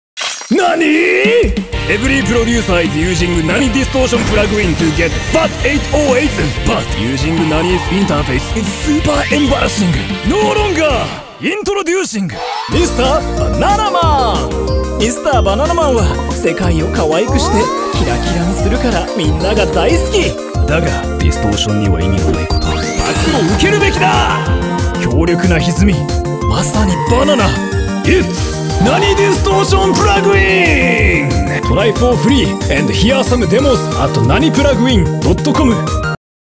NANI DISTORTION PLUGIN : BANANA
!! NANI DISTORTION PLUGIN !!

Although NANI may look ridiculous on the outside, underneath is a high powered distortion plugin.